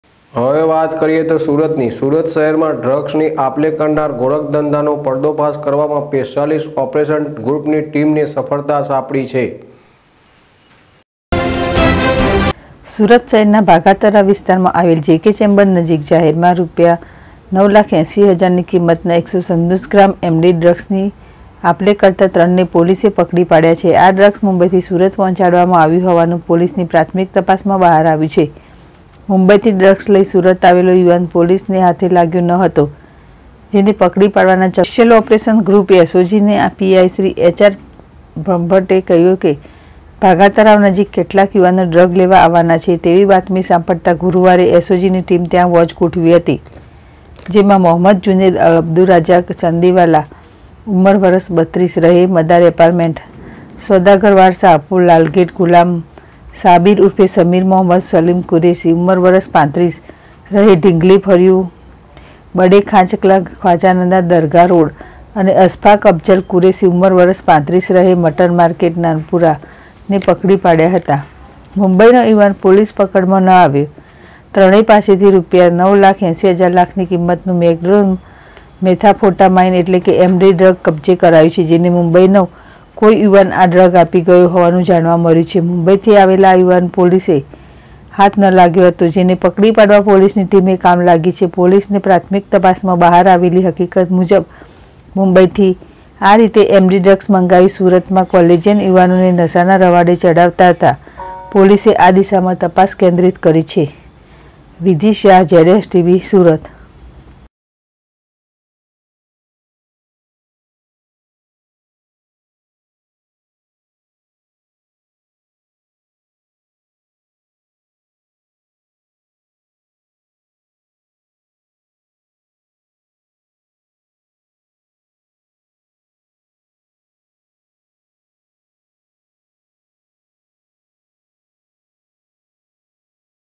સુરત શહેરમા ડ્રગ્સ ની આપલે કરવાના ગોરખધંધાનો પર્દાફાશ કરવામાં // ZSTVNEWS//
સુરત શહેરમા ડ્રગ્સ ની આપલે કરવાના ગોરખધંધાનો પર્દાફાશ કરવામાં